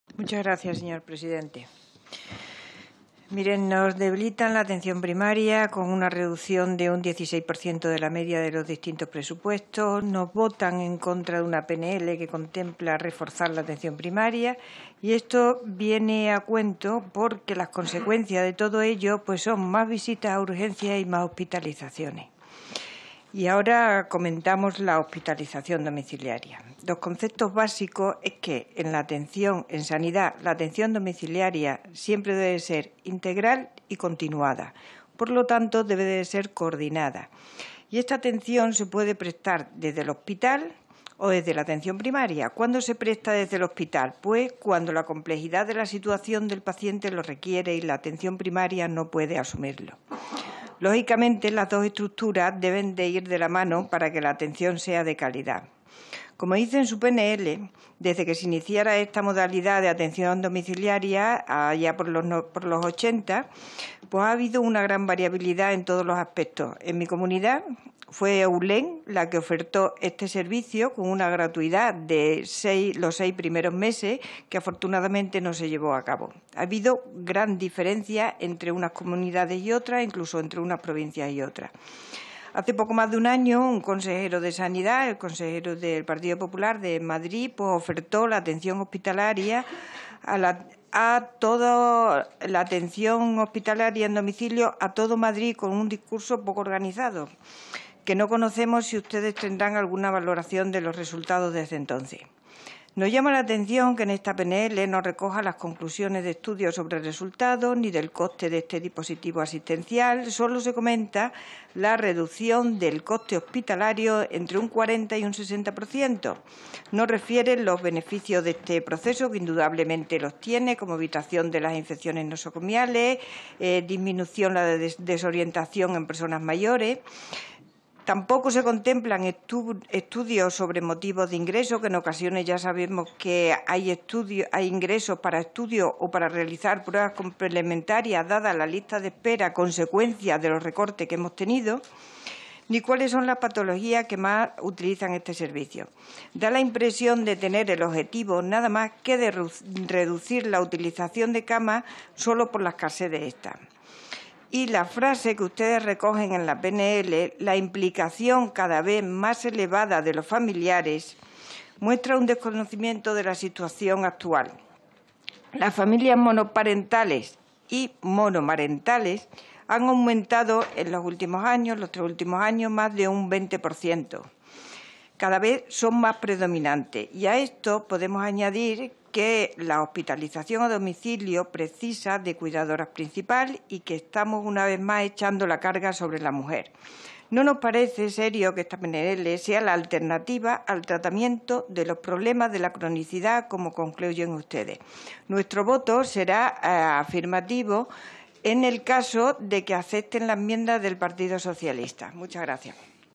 Amparo Botejara en la Comisión de Sanidad, Consumo y Bienestar Social el 28 de junio
En el debate de la PNL relativa a la hospitalización domiciliaria.